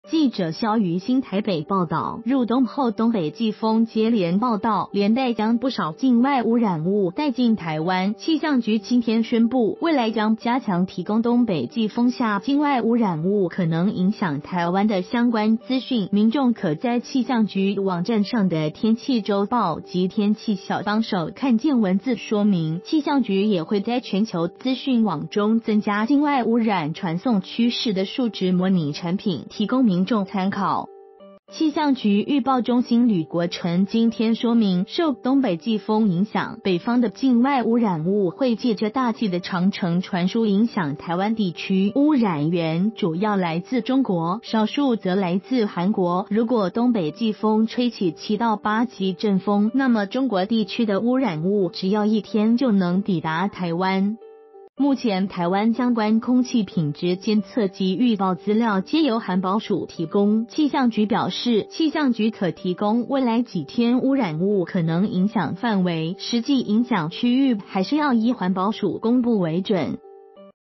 空汙情況嚴重 氣象局新增汙染物趨勢模擬
〔記者蕭玗欣／台北報導〕入冬後東北季風接連報到，連帶將不少境外汙染物帶進台灣，氣象局今天宣布，未來將加強提供東北季風下境外汙染物可能影響台灣的相關資訊，民眾可在氣象局網站上的天氣週報及天氣小幫手看見文字說明，氣象局也會在全球資訊網中增加境外汙染傳送趨勢的數值模擬產品，提供民眾參考。